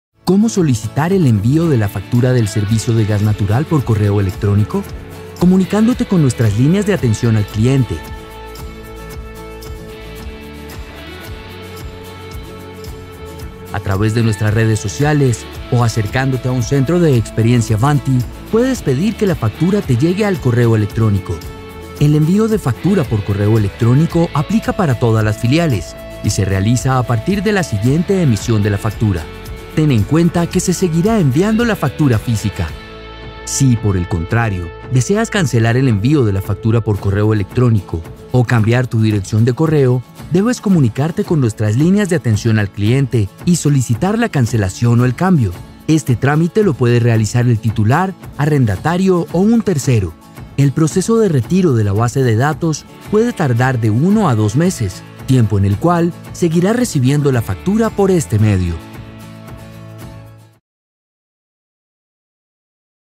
¿Cómo solicitar el envió de la factura gas natural domiciliario por correo electrónico?
Ahora solicitar tu factura de gas natural domiciliario por correo electrónico, es una realidad, comunícate con nuestras líneas de atención al cliente o acércate un centro de experiencia Vanti.
Línea de atención Bogotá : 307 8121
Línea de atención Santander 654 8399
Línea de atención Cundinamarca, Boyacá y Cesar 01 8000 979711